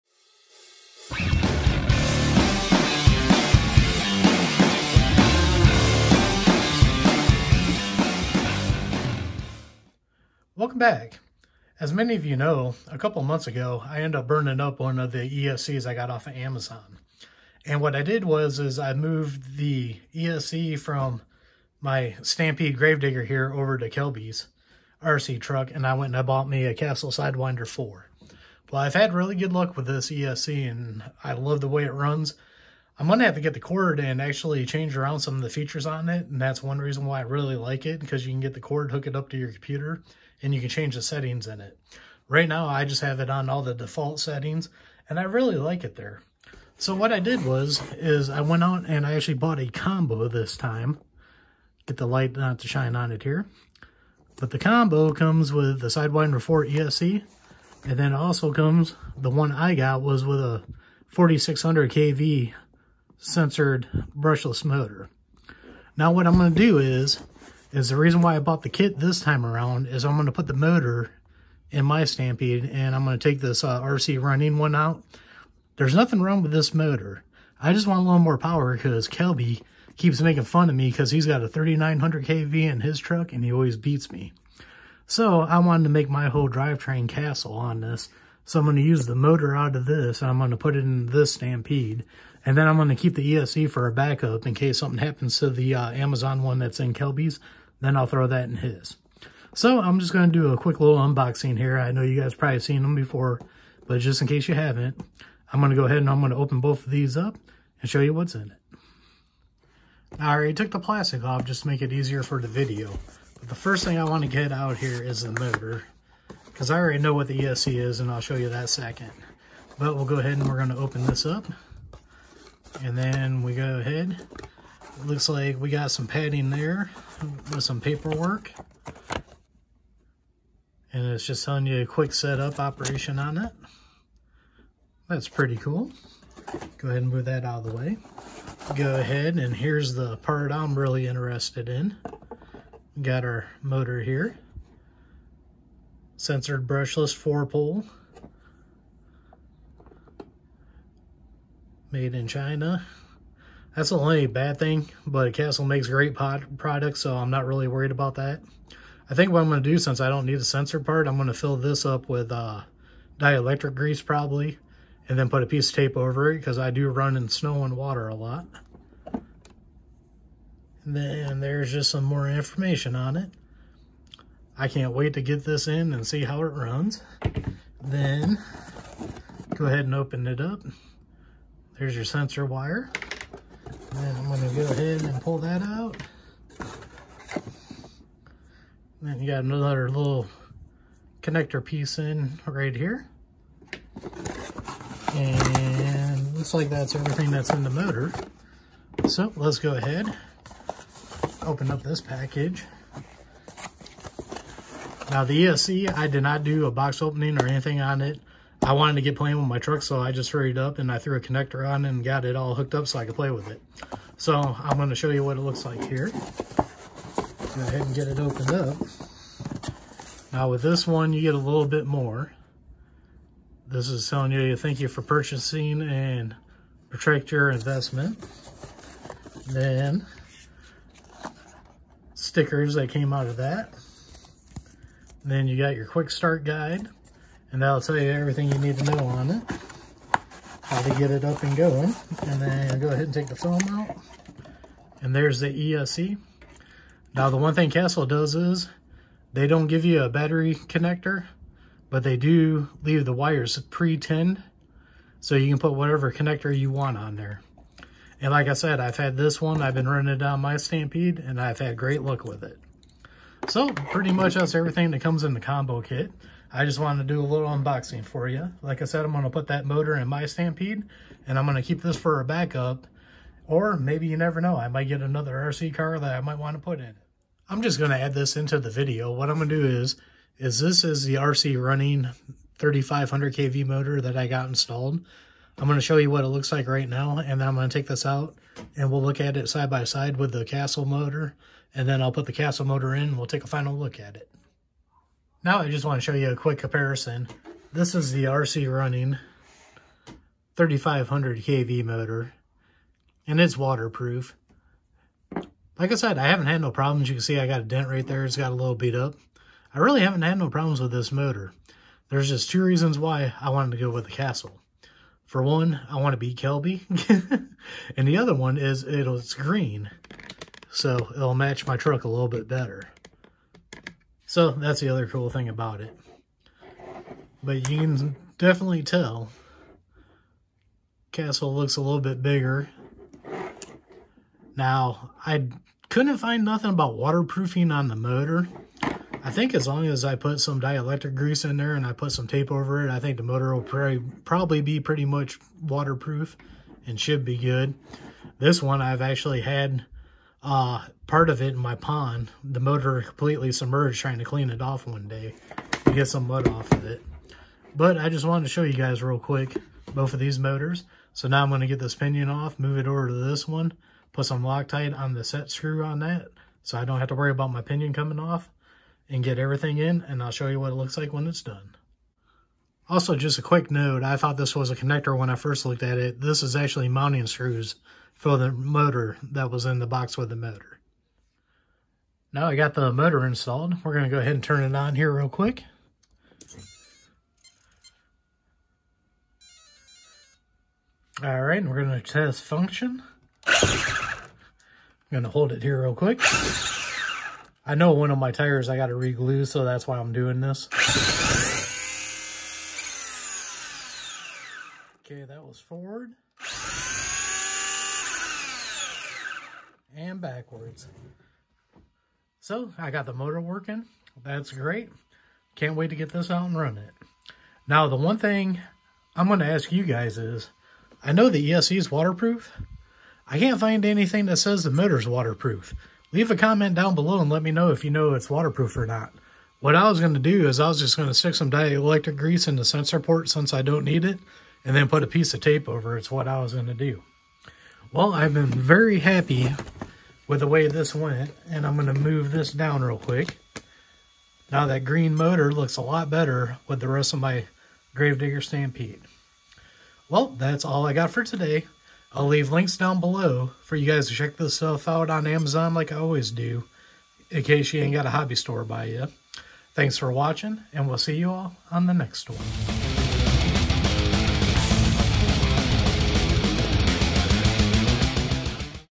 Castle Creation Sidewinder 4 RC Esc & Motor Unboxing
Castle Creation Sidewinder 4 RC Esc & Motor Unboxing. Doing an unboxing and taking a look at the Castle Creations sidewinder 4 RC ESC and 4600kv motor combo. Also showing you them in the vehicles. This is a great upgrade for your RC car, RC truck, or RC buggy.